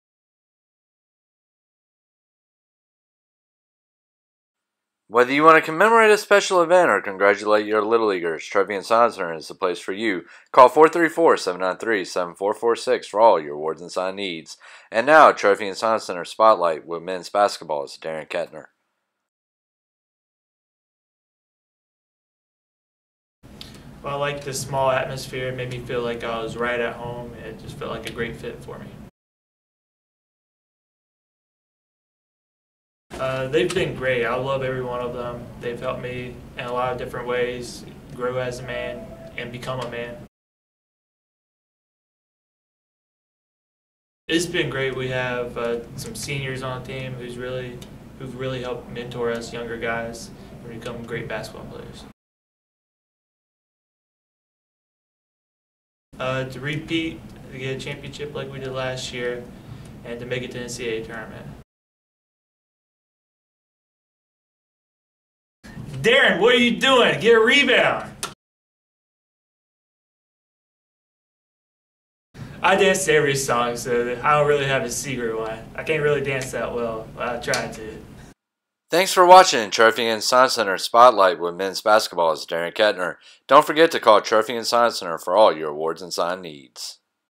Trophy and Sign Center Spotlight: Men's basketball's Darin Ketner
Averett student-athletes are put in the spotlight each week with random questions.